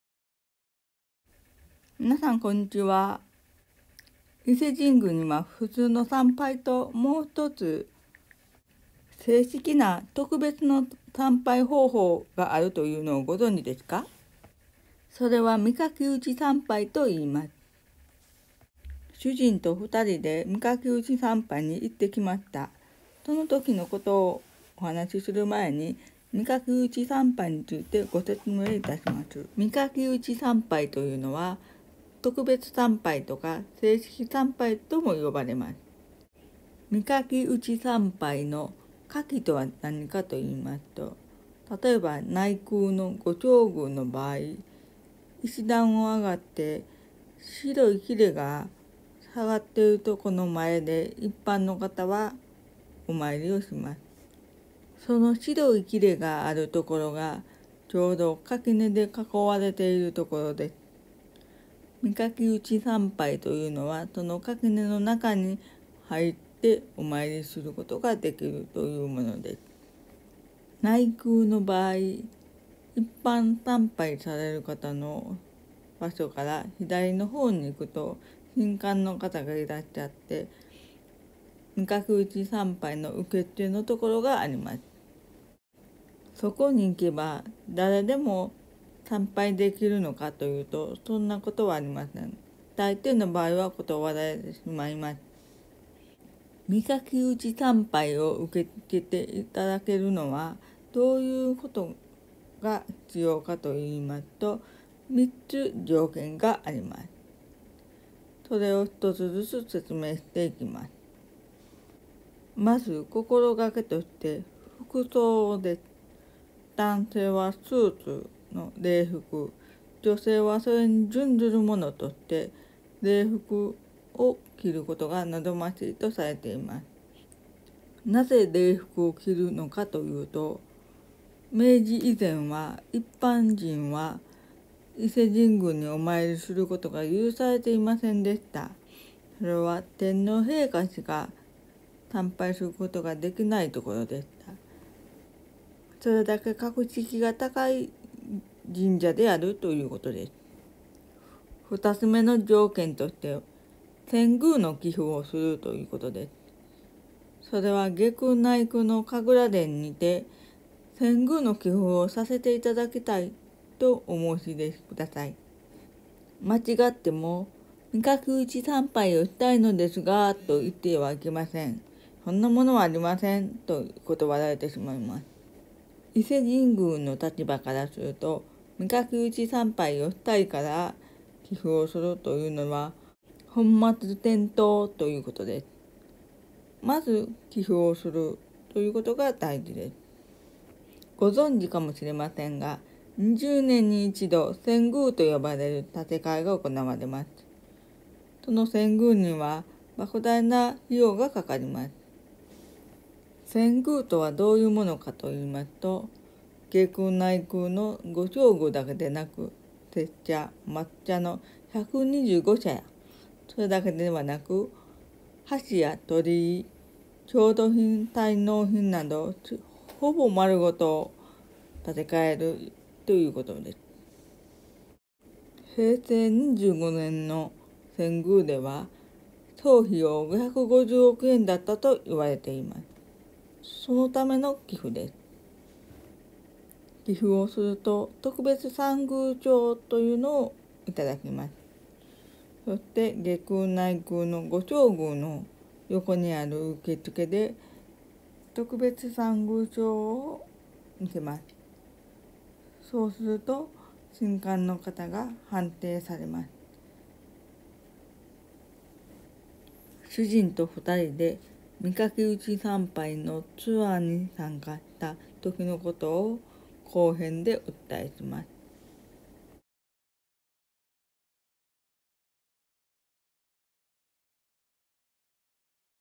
伊勢神宮の正式参拝方法、御垣内参拝を知っていますか？前篇 漫画解説 実話 参拝動画
☆メールマガジンまぐまぐご登録はこちら

伊勢神宮の特別な参拝方法、「御垣内参拝」の内容と、参拝の方法をお伝えします。

タイム
0:17 御垣内参拝の説明
1:30 服装の決まり
2:29 遷宮
4:01 寄付

▼オンデマンドの書籍と電子書籍kindleを出版しています。